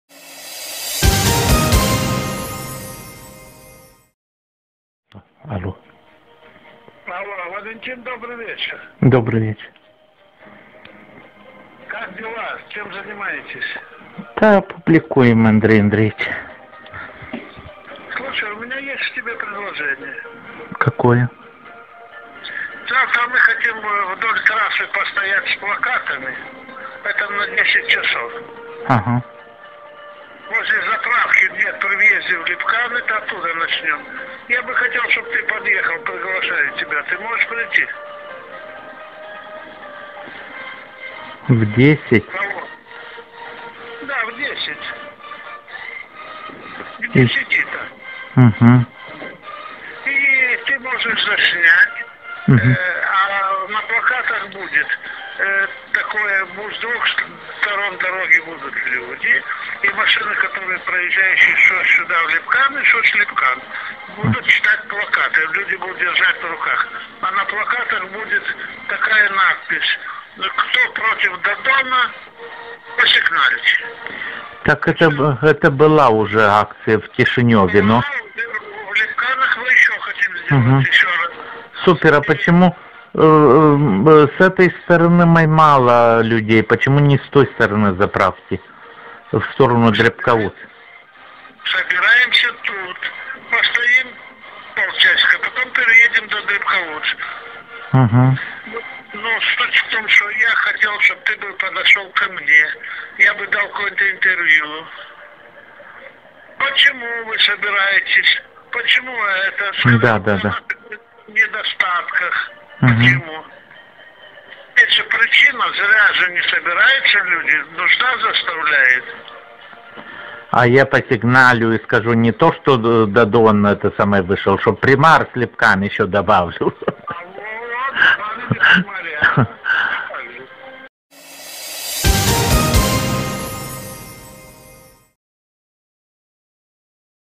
SILA V PRAVDE ?
Семен Гринчук LIDERUL PN Lipcani scrie aiureli de mine si am cautat o ora in telefon ca sai demonstrez ca eu am fost invitat, nu am venit din capul meu la flasmobul de la Lipcani Хочу дать опровержение словам, высказанных в посте Valentin Borodachi
23 августа состоялась акция "Просигналь кто против Додона",в которой я лично учавствовал.Во время акции подъехал гражданин Valentin Borodachi ,которого никто не приглашал и не ждал,он снимал/фотографировал процесс акции и брал у нас интервью.Это было его личное добровольное желание-НИКТО ИЗ УЧАСТНИКОВ АКЦИИ И ОРГАНИЗАТОРОВ НЕ ПРЕДЛАГАЛ ДЕНЕГ В РАЗМЕРЕ "50 ЛЕЙ" гражданину Valentin Borodachi.Вопрос о деньгах вообще не поднимался!
Поэтому "уважаемый" гражданин Valentin Borodachi-СТЫД И ПОЗОР ВАМ ЗА ТАКИЕ ЛЖИВЫЕ СЛОВА, а если вы рассчитывали,что вам заплатят за вашу съёмку (ещё раз уточню это было вашим добровольным решением),то надо было обратиться ко мне,я бы вам от себя лично заплатил "нужную" сумму! Ascultati cum seara la 21-55 ma invita la acest flesmob dar mai sus scrie aiureli,minciuni...ca nu m-au invitat Asa ca "СИЛА В ПРАВДЕ " de data asta a-ti pierduto.Acum ceretiva scuze in public sa vad ce fel de "СИЛА В ПРАВДЕ "СТЫД И ПОЗОР ВАМ ЗА ТАКИЕ ЛЖИВЫЕ СЛОВА, aveti - Tos.TV